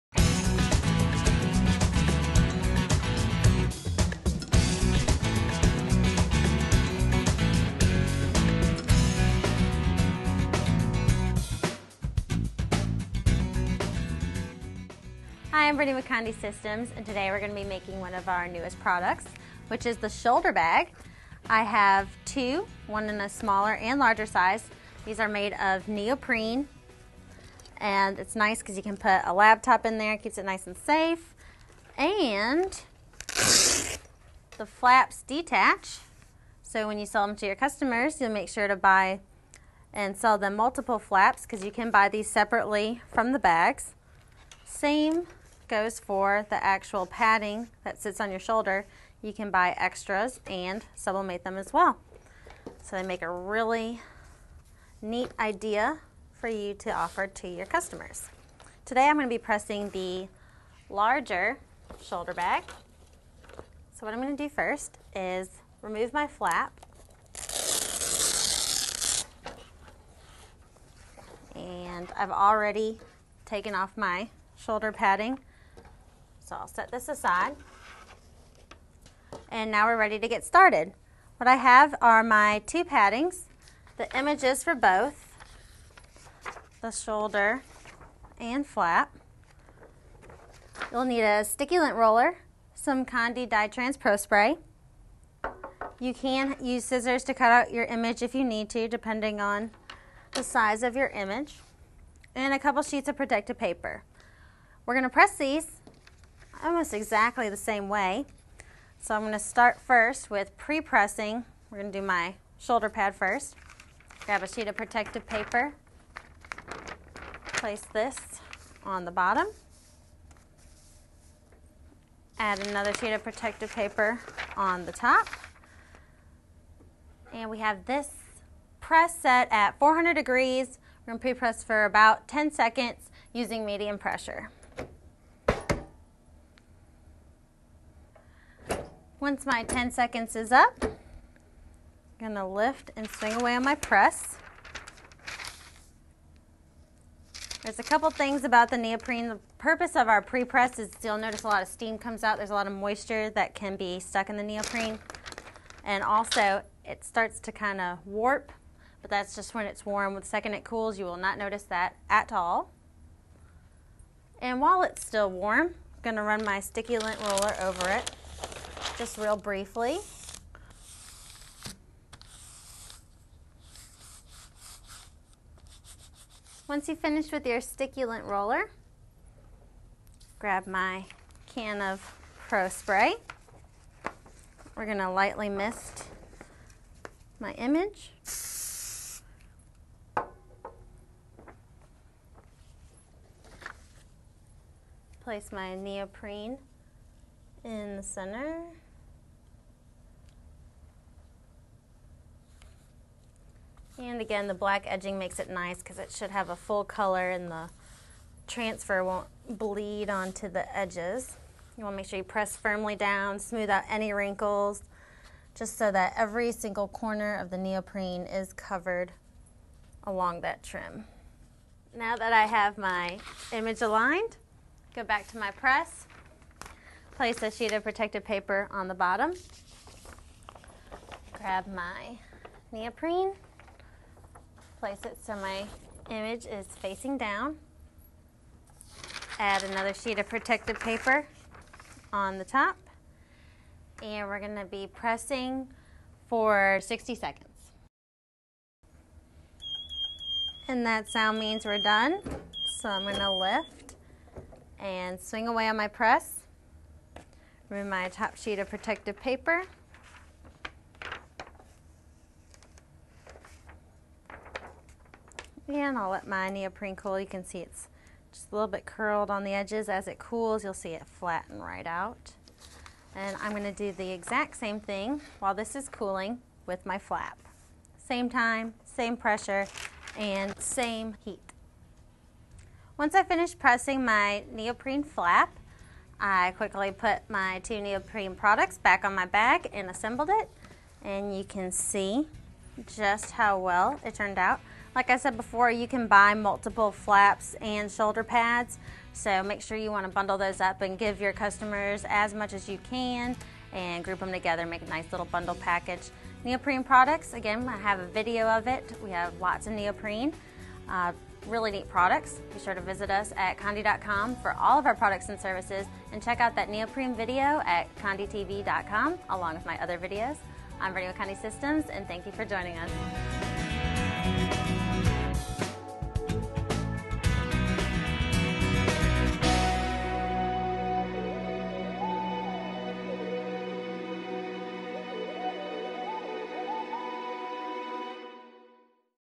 How to Sublimate Shoulder Bags - Step-by-Step - Conde Systems, Inc.
Using a George Knight® DK20S™ Heat Press, Condé's Brittany Anderson demonstrates the procedures for applying dye sublimation transfers to the new neoprene shoulder laptop bags. The part number for these bags is MP046 and MP047 The transfers are created using a Ricoh® GX7000 printer with Sawgrass® SubliJet-R™ sublimation inks on TexPrint-R® Sublimation Paper, with a George Knight™ heat press. All products are available from conde.com, including the heat press used in the demo.

This video applies to Conde part numbers: MP046, MP047.